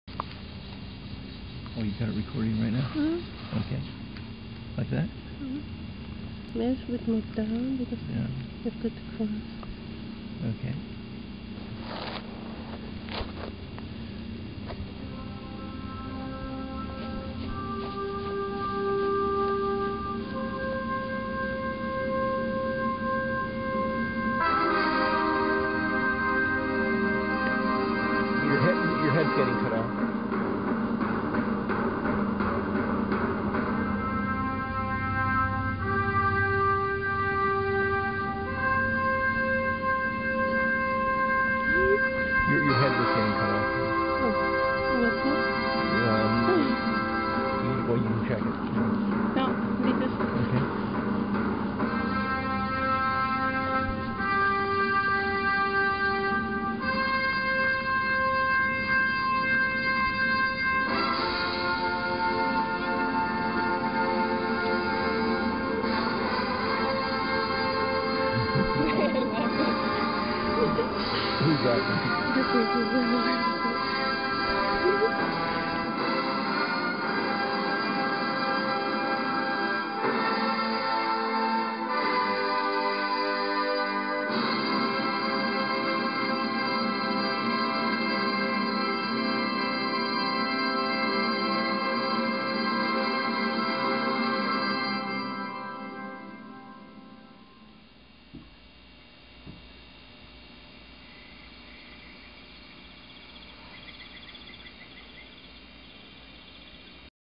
The Inpex Dance with MewFushisDad
in front of the Anti-Virus Norton

Summer house. Let´s swing.

Thanks for the rides on the Tandem and

Triplet.